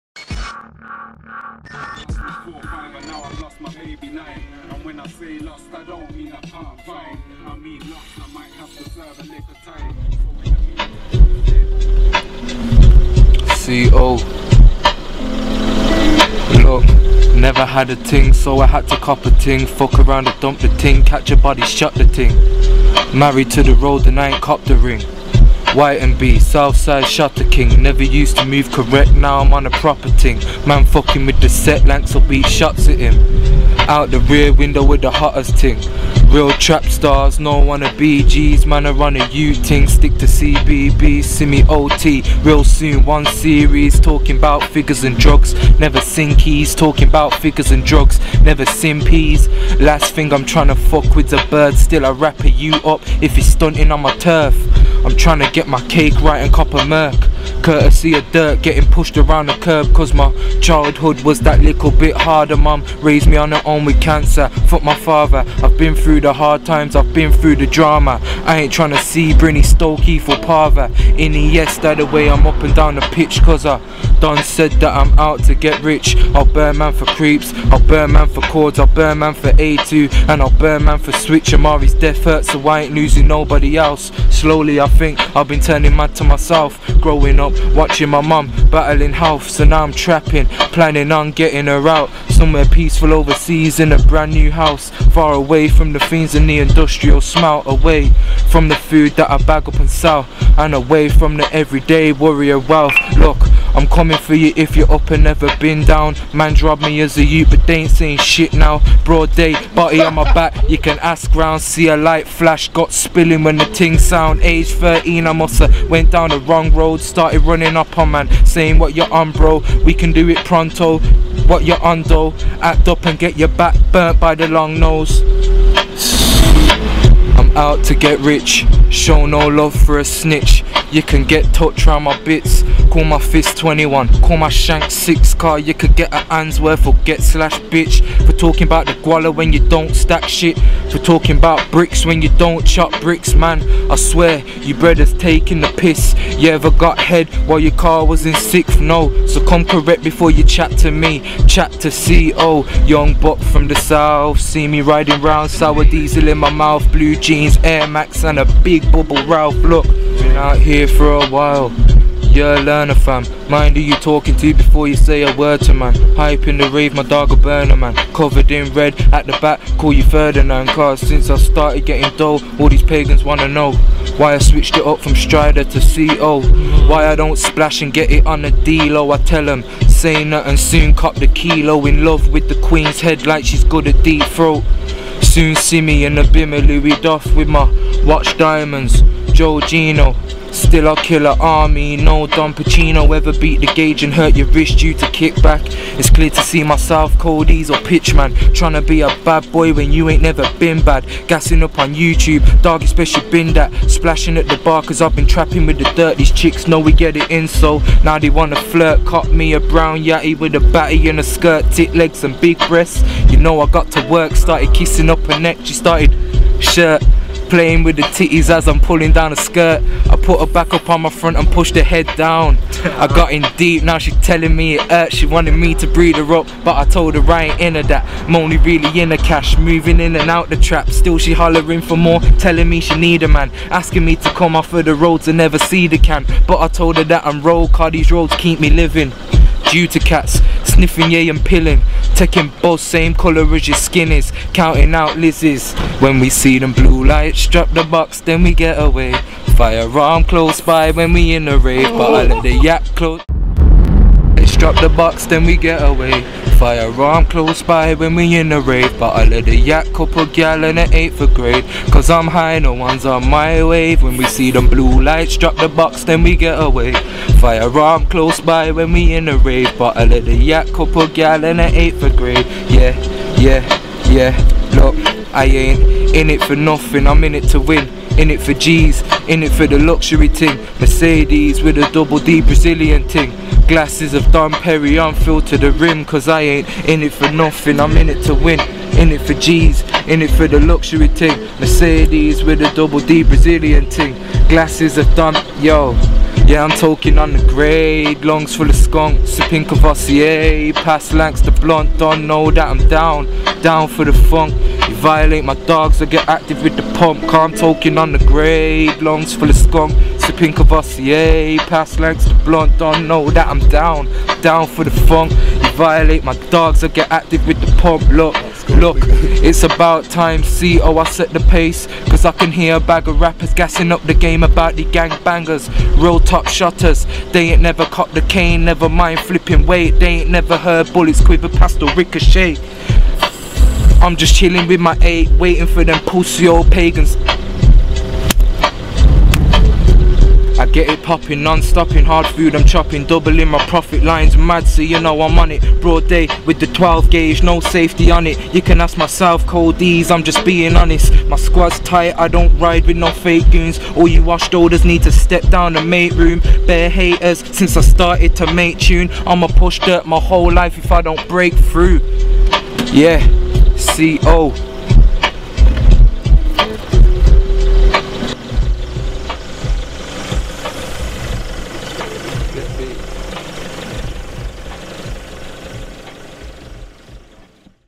'Visual Verses' Ft. Strider - C.O
Southsides' Strider - C.O rolls up to drop his part for the 'Visual Verses' series.. after his debut on p110 he comes with a next batch of FIRE!